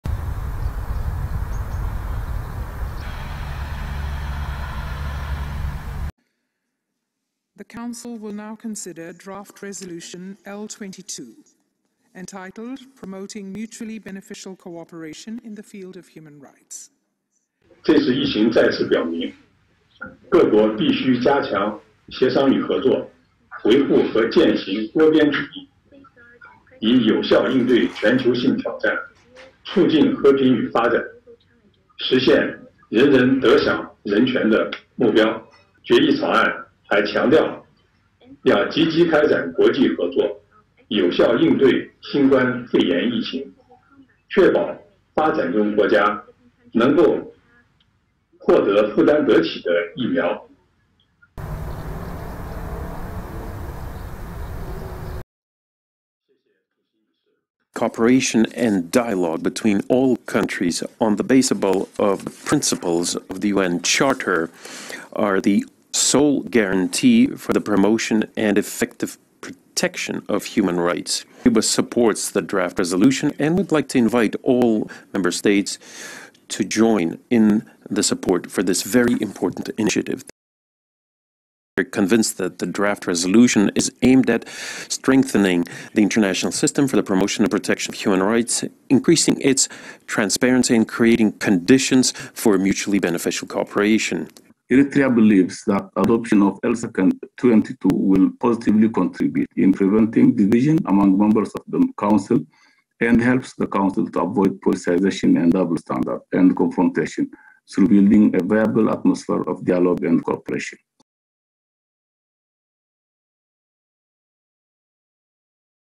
GLOBALink | UN rights body adopts China-sponsored resolution on human rights cooperation
The UN Human Rights Council on Tuesday adopted a China-sponsored resolution, in which states and other stakeholders are called upon to undertake a cooperation approach in the field of human rights.